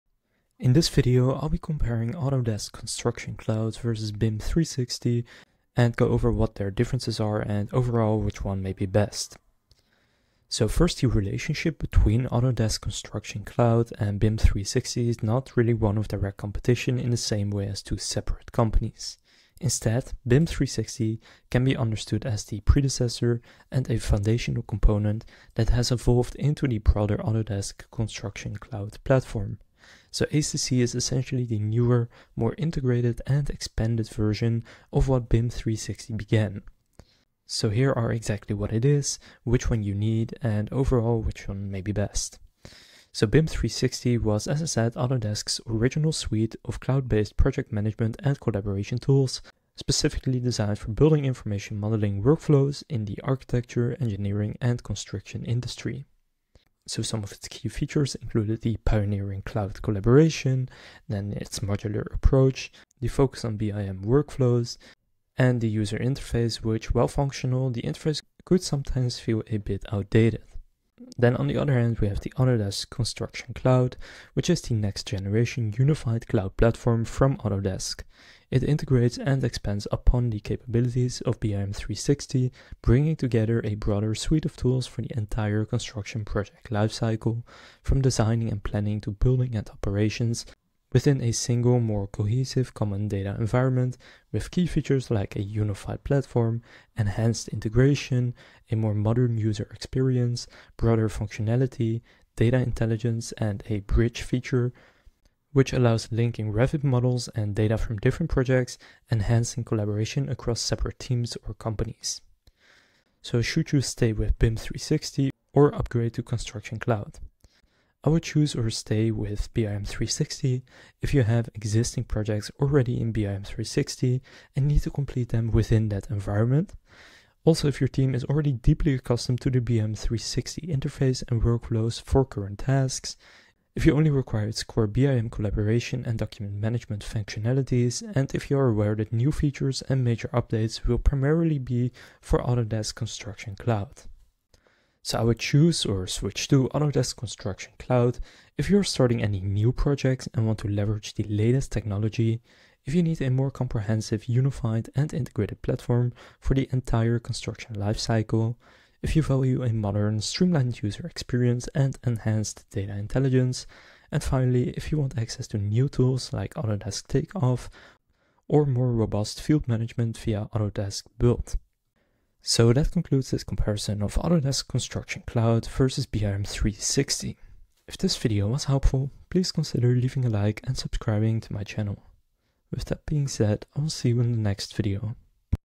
Autodesk Construction Cloud vs BIM 360 (2026) | Which One is Better?
In this video I'll compare Autodesk Construction Cloud vs BIM 360.